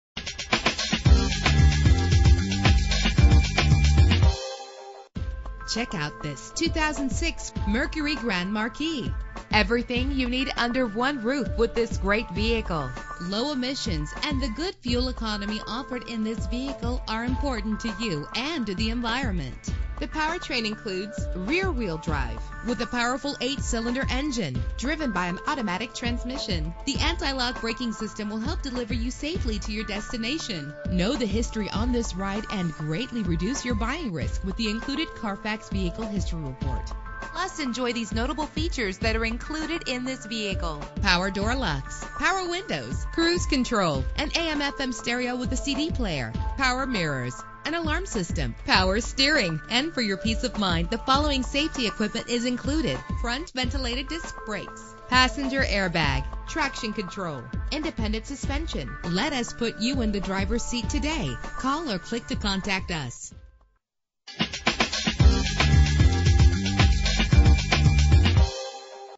2006 Mercury Grand Marquis - Totowa NJ
Year: 2006
Make: Mercury
Model: Grand Marquis
Trim: GS
Engine: 4.6 liter 8 cylinder 16 valve
Transmission: 4-Speed Automatic
Color: Light Green
Mileage: 61576
Address:
440 Route 46 East